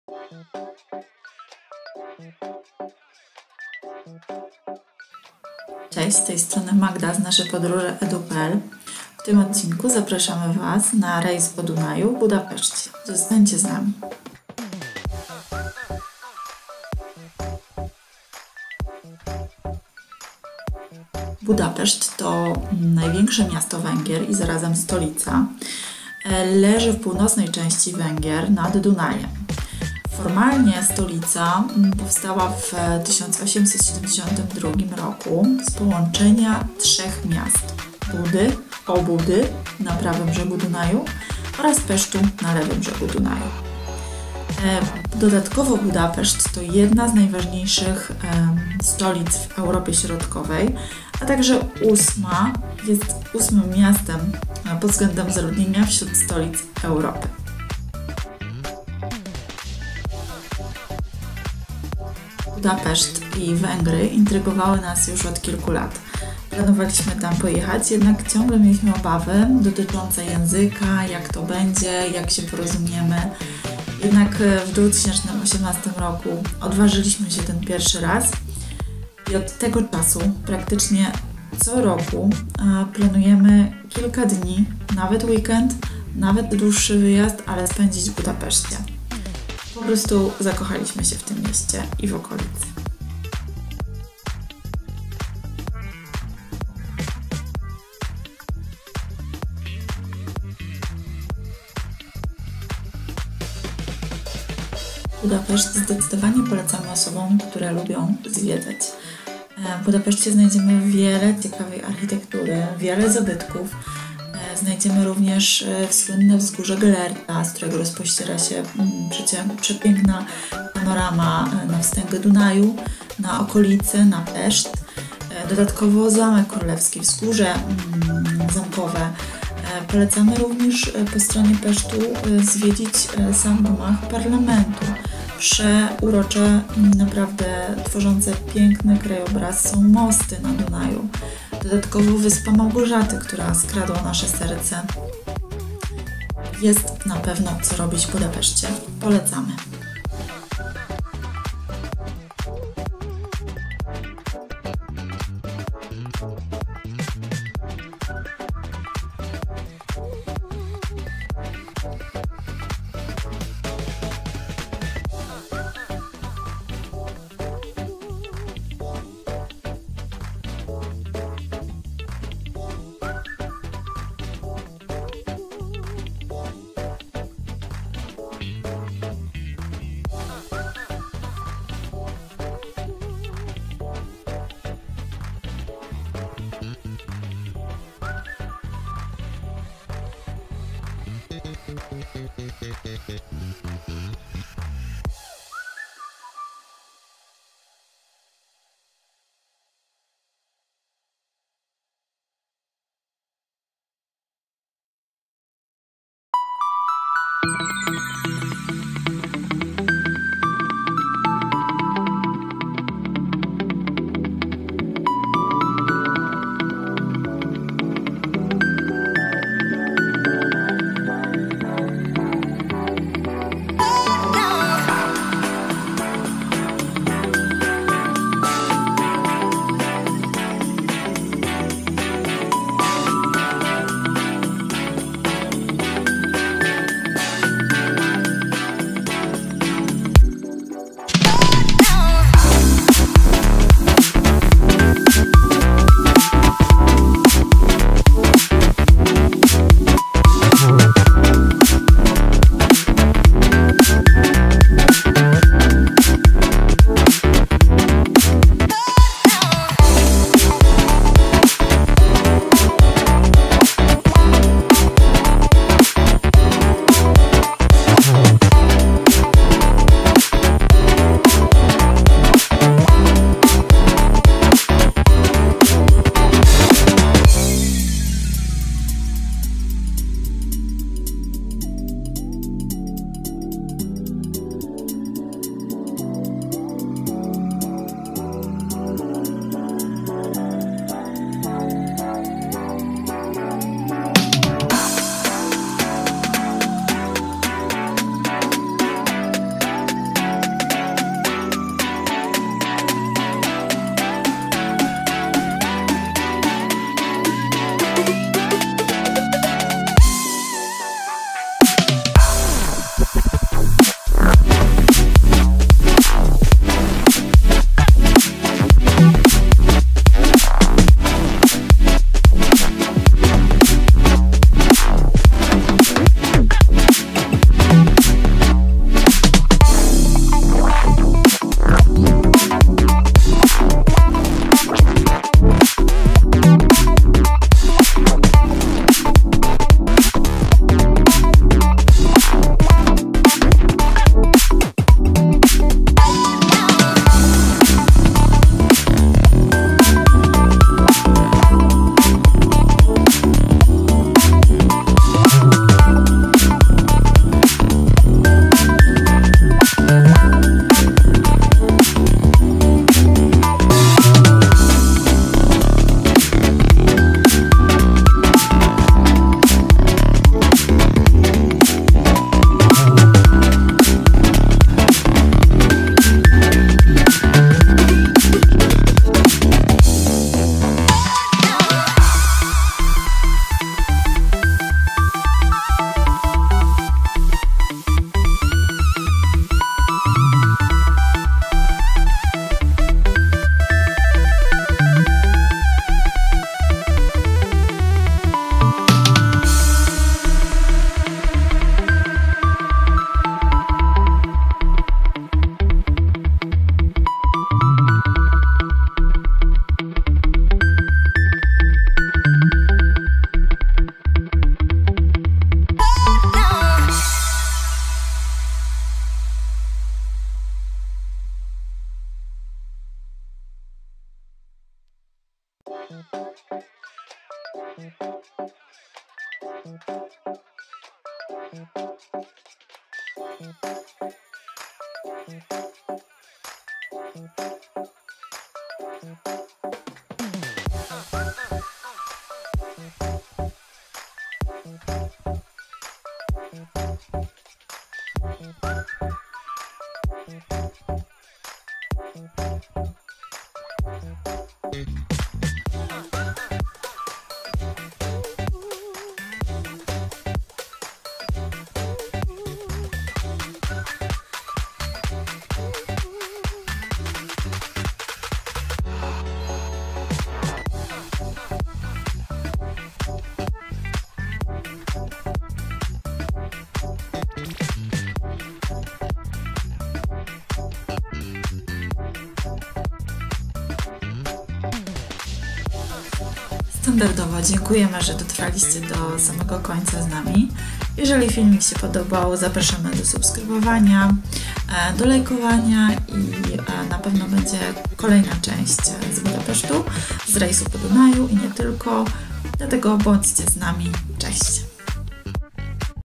WĘGRY - BUDAPESZT Rejs po Dunaju, vlog 1
W tym odcinku zabieramy Was na Węgry a dokładnie do Budapesztu, na rejs po Dunaju. Naszym zdaniem jest to najciekawsza i nieśpieszna atrakcja, którą warto wypróbować, jeśli ktoś lubi pływać :)
Zniżka Airbnb! Zarejestruj się za pomocą naszego linku, a otrzymasz nawet do 250 zł zniżki na swoją pierwszą podróż